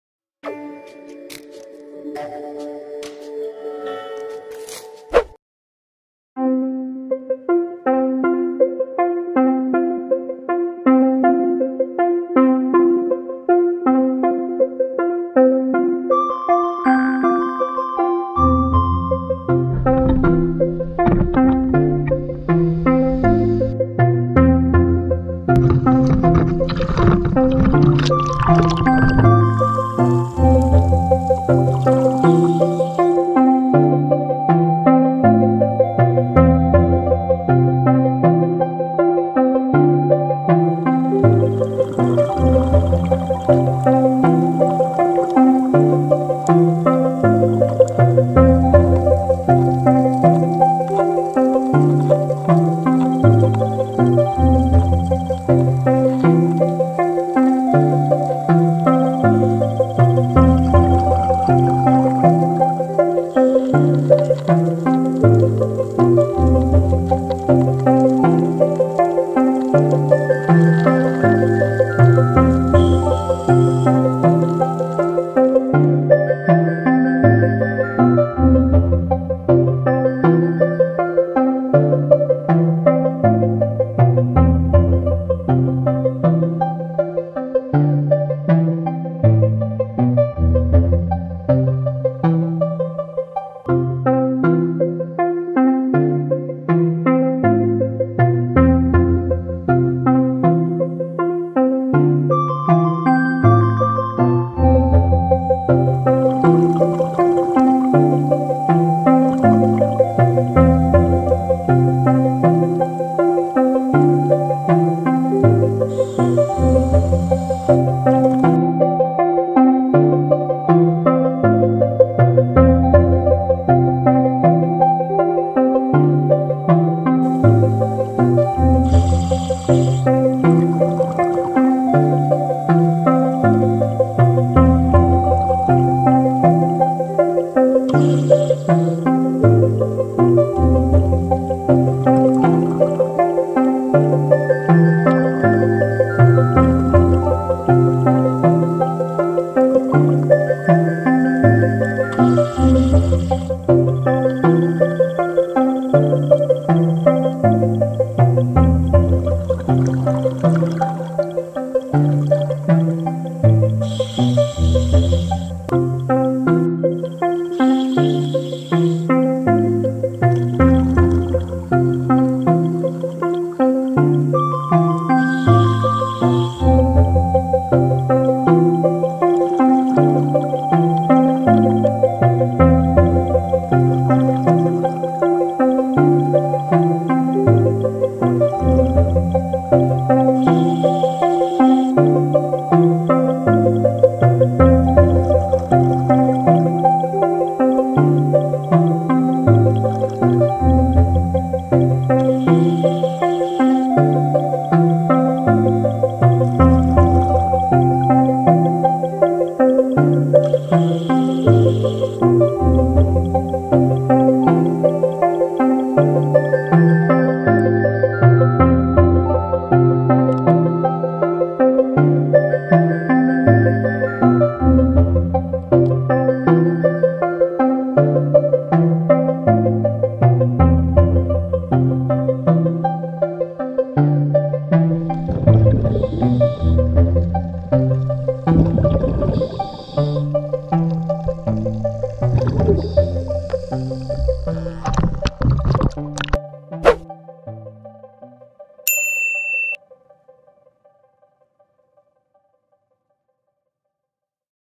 How Small this Nudibranch ? Shaun the Sheep Nudibranch
One of the beautiful Nudibranch  I found , just diving on shallow site can found this , and this video also I will show you the size of this nudibranch